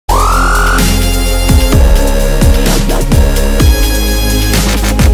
My New Intro~
No character in particular, just a random thing I animated to intro-ize XD